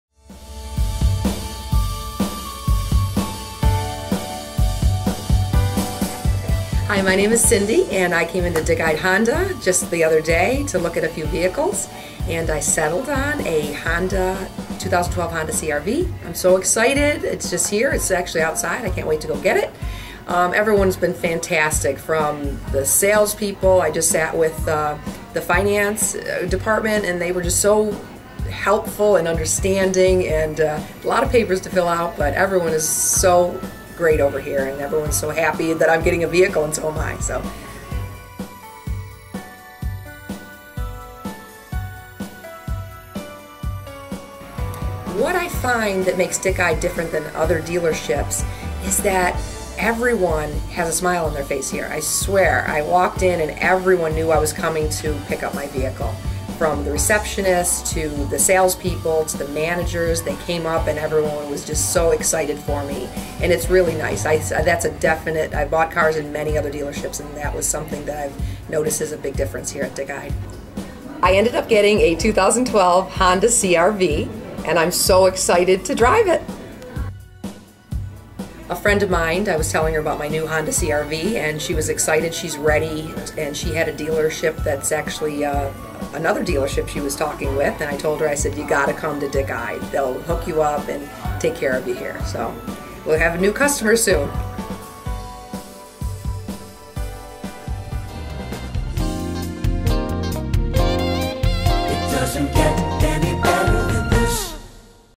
Customer Testimonial: Cindy - 2012 Honda CR-V- Dick Ide Honda, Rochester NY Dealer Review
Cindy recently came to Dick Ide Honda to pick up her brand new 2012 Honda CR-V! See what she has to say about her experience!